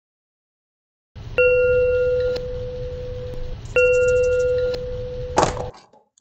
Rabbit Rings the Bell for Carrots
Made with my (Roonie's) time on the bus in Procreate
Character design by Mil/Lach/Rex